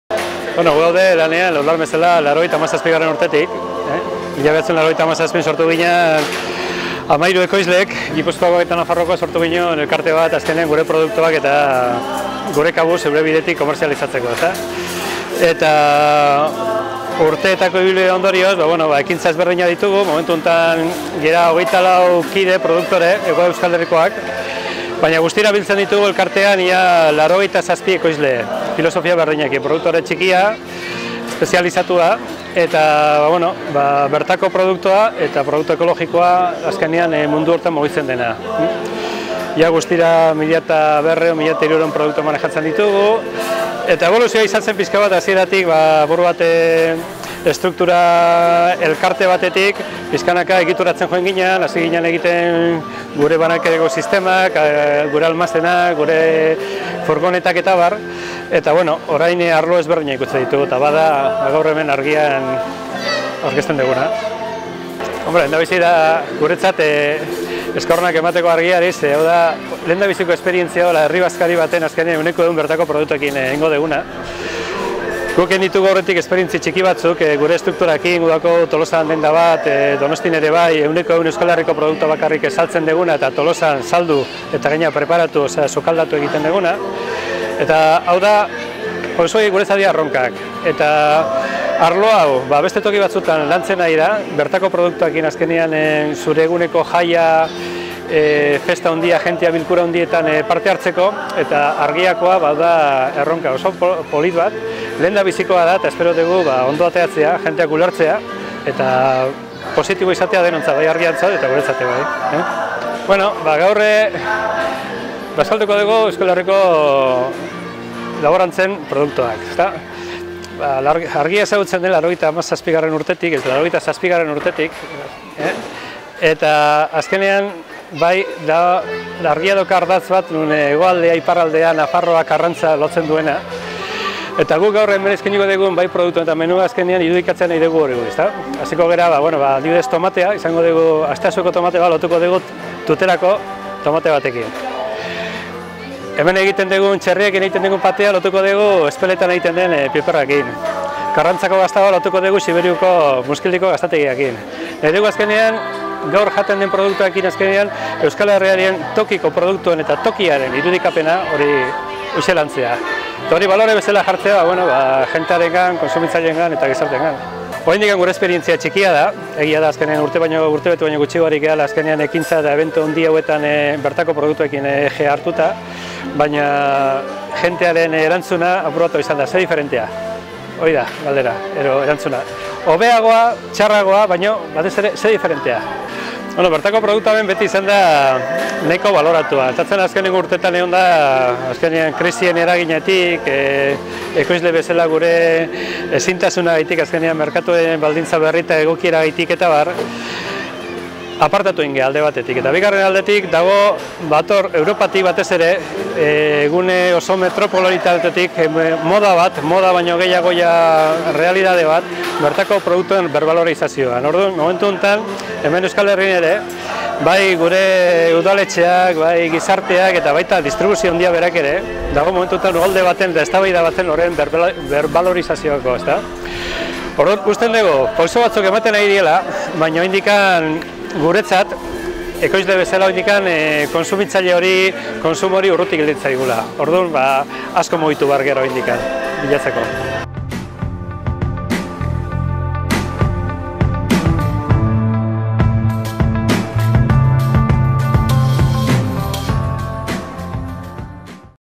Herri bazkaria bertako produktuekin 100% egina
ARGIA Eguneko herri bazkaria bertako produktuekin osatu genuen.Lur Lan kooperatibako arduradun Joseba Beitiak Euskal Herriko produktuen alde garatzen ari diren lana azaldu digu.